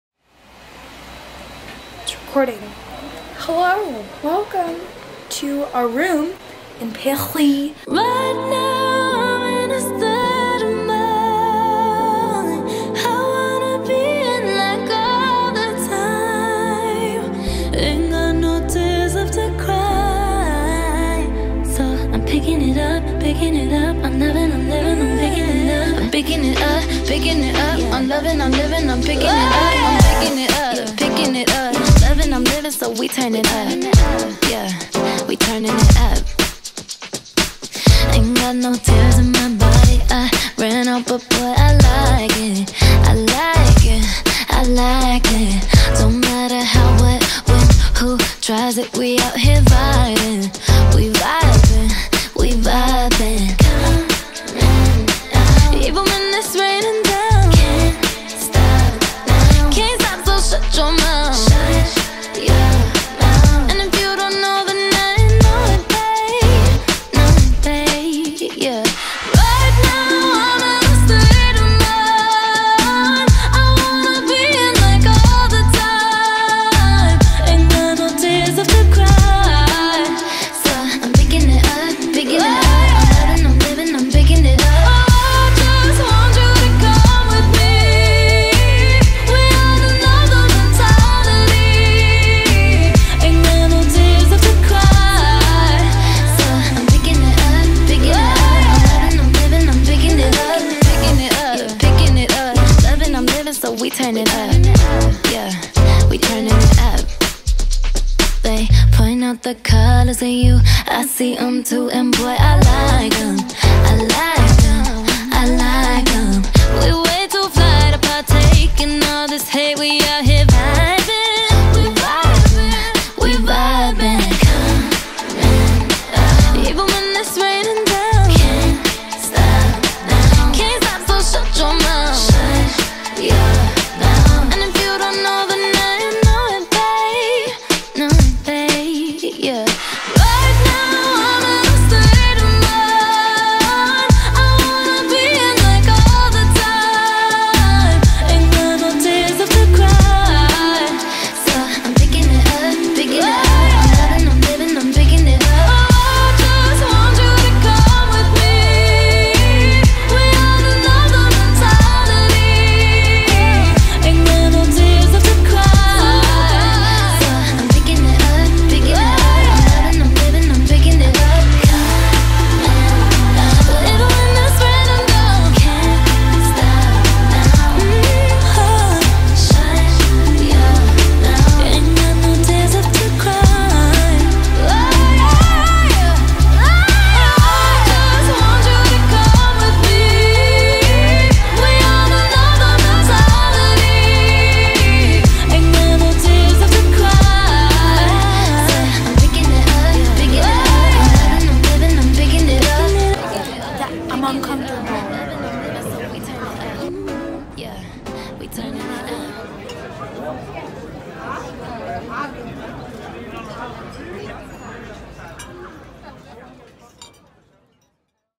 Up A Notch
This is our whirlwind four day trip to Paris over spring break.
Make sure and skip the line at the Arc de Triomphe and the Louvre!